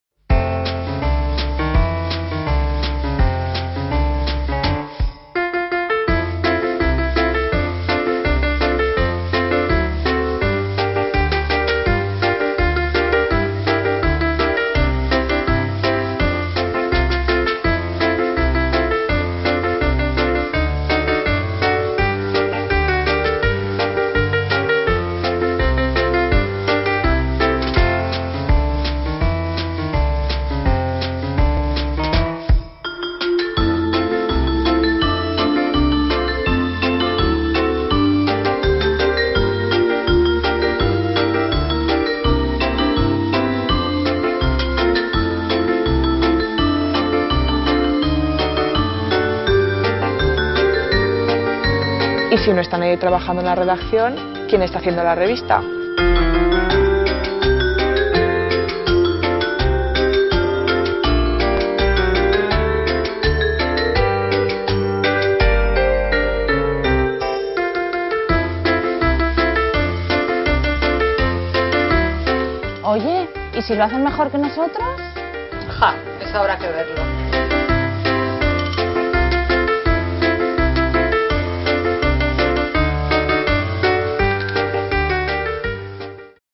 ¿Qué está pasando en YO DONA?
YO DONA siempre ha buscado convertir a sus lectores en protagonistas. Y ahora esa apuesta alcanza una nueva dimensión: en esta ocasión tú haces la revista. Puedes elegir contenidos, personajes y temas. ¿Cómo? conviértete en un profesional del periodismo y, además, ¡puedes ganar un viaje a la Toscana.

Pero... ¿Y qué hacen los miembros de la rección? Pues juegan a las cartas, hacen punto, algunas 'matan marcianos', se pintan las uñas, cocinan, toman el sol... Y en definitiva, dejan pasar el tiempo mientras esperan a tener en sus manos la revista ¡hecha por ti!